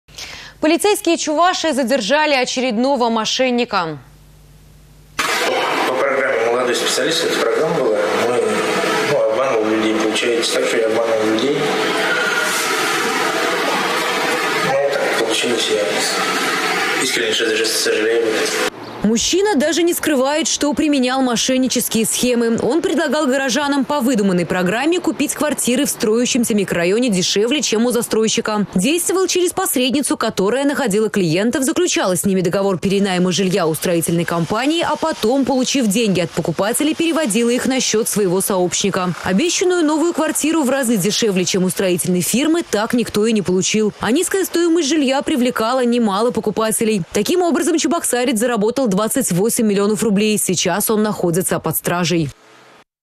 Полицейские Чувашии задержали очередного мошенника
Мошенник предлагал горожанам по выдуманной программе купить квартиры в строящемся микрорайоне в разы дешевле, чем у застройщика. Низкая стоимость жилья привлекла немало покупателей. Обещанную новую квартиру  так никто и не получил. Таким способом чебоксарец заработал 28 миллионов рублей. Сейчас он уже находится под стражей.